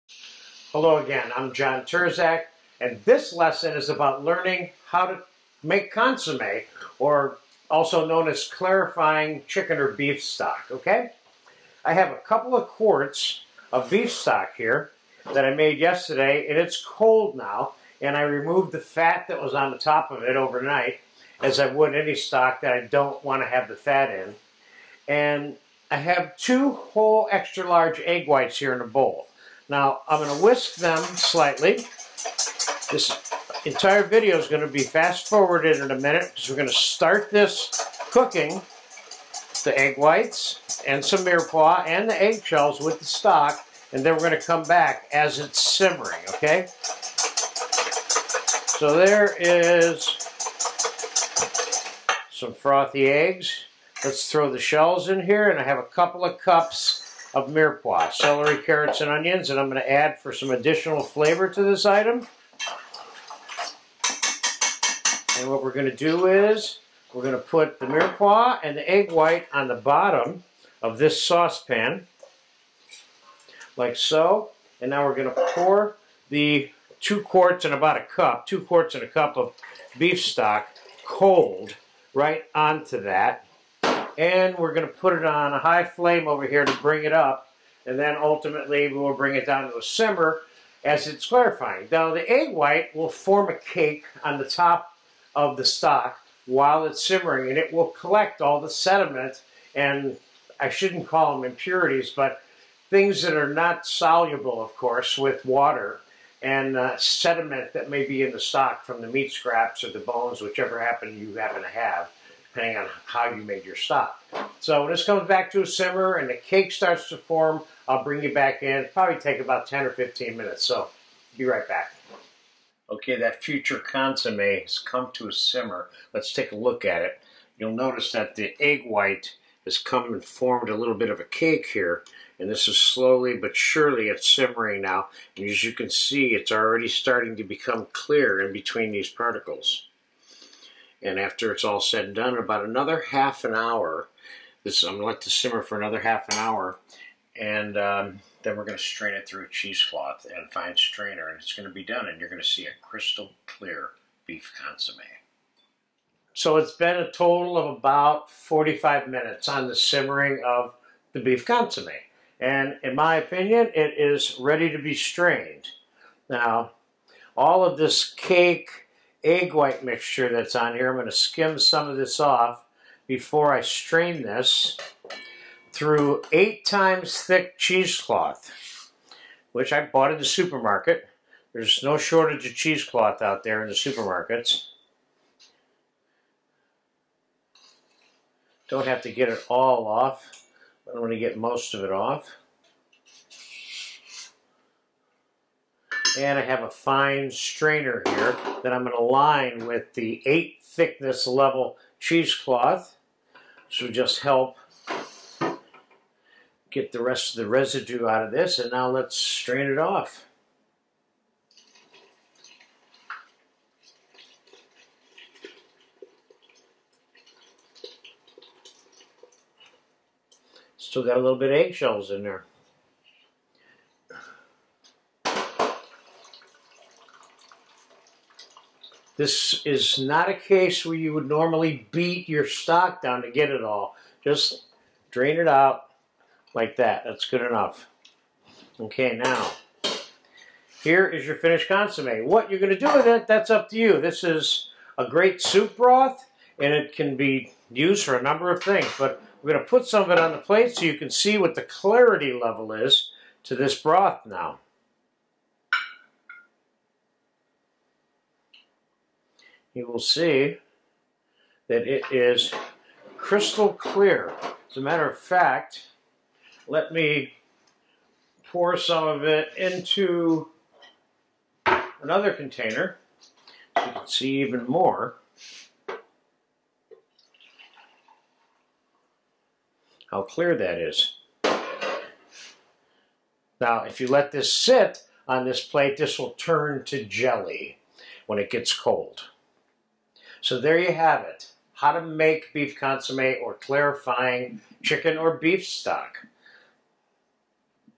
Beef Consommé
This video demonstrates the basic French method for clarifying a stock.  Typically, beef, chicken, fish, other fowl and game qualify as stocks that can be clarified.   Unlike the use of the term “clarified butter” which denotes the removal of water and casing solids from the butter fat, clarifying stock denotes the removal of fine particles of food and/or fat within the stock that causes it to be cloudy.  The uses of consommé are usually rooted in the aesthetic visual uses. Once it’s clarified it is considered to be nicer looking and is then used in a variety of different preparations that partially rely on the clearness of the consommé as part of its presentable value.  Other than those purposes just mentioned, the consommé is also traditionally reduced to a fairly concentrated flavor and used as a cold jelly (gelatinous aspic) for soups or for decorating showpiece preparations or to simply add the flavor of the stock in the form of a coated jelly to any number of different individual food preparations.  Good examples of that would be a lunch portion of cold salmon fillet coated with aspic and garnished with aspic coated egg or caviar(s). Traditionally small or large whole poached fish are often coated with consommé and in many cases additional gelatin is added to the consommé to strengthen the texture of the aspic itself, often times to preserve the foods longer in the gelatin while also adding flavor.  The reduced consommé is also poured into or used as a topping for a variety of different meat pates.